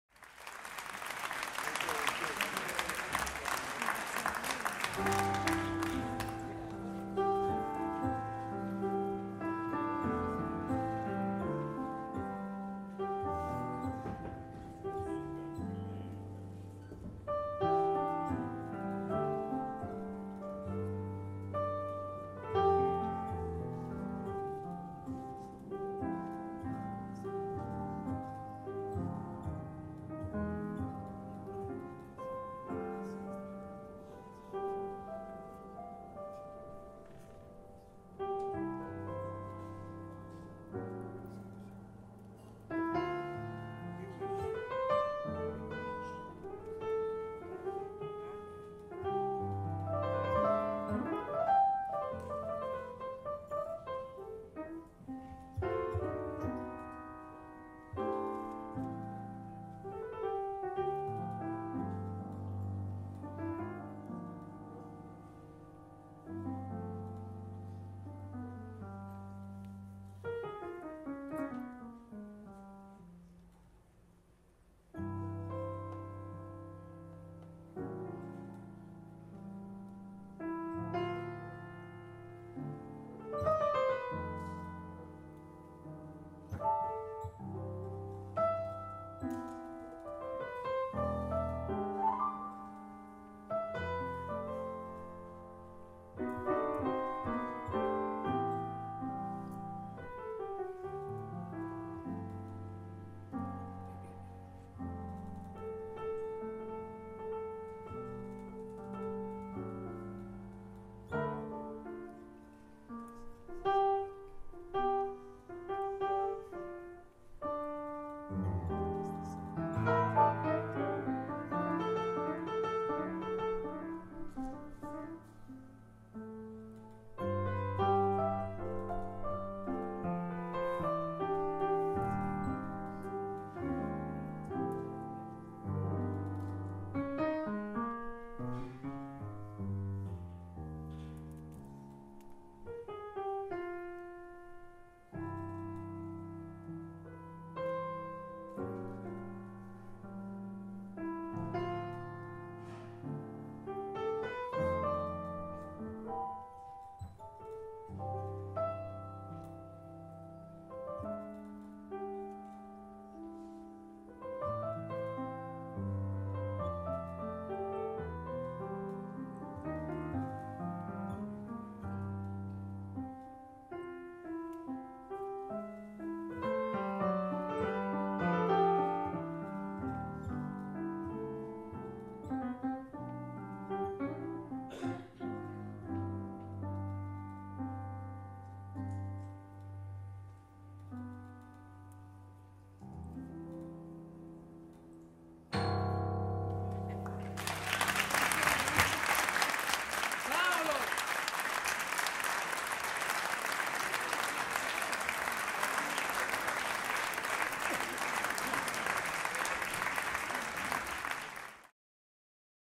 Enrico Intra piano solo a Villa Simonetta
Enrico Intra in piano solo a Villa Simonetta Milano il 10 luglio 2014